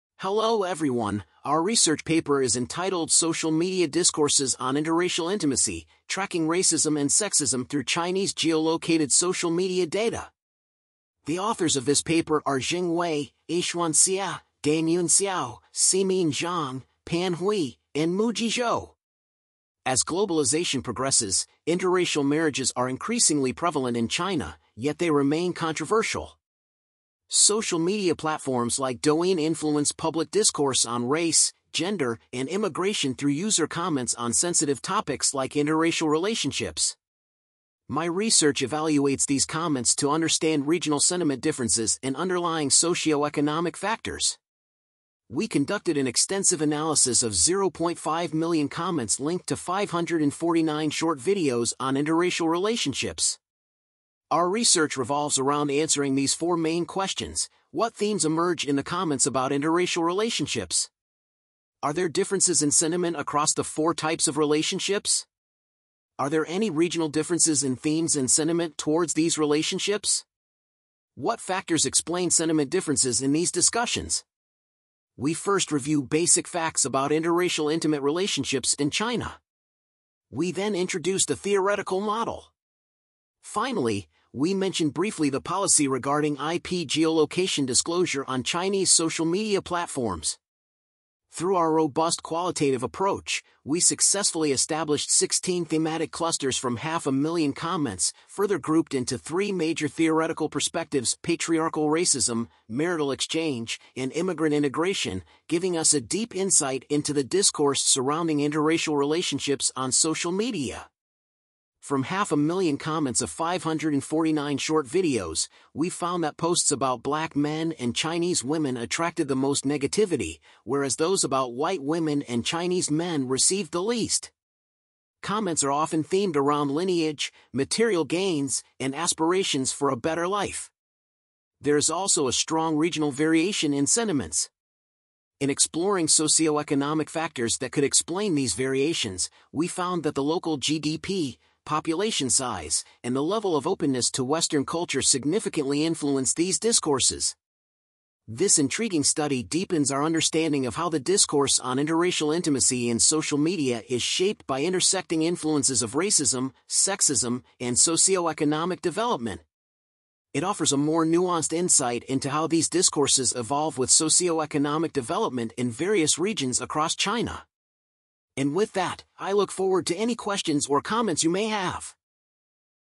[rfp0124] Social Media Discourses on Interracial Intimacy: Tracking Racism and Sexism through Chines
"Social Media Discourses on Interracial Intimacy: Tracking Racism and Sexism through Chinese Geo-located Social Media Data

Zheng WEI, Yixuan Xie, Danyun XIAO, Simin Zhang, Pan Hui, Muzhi ZHOU"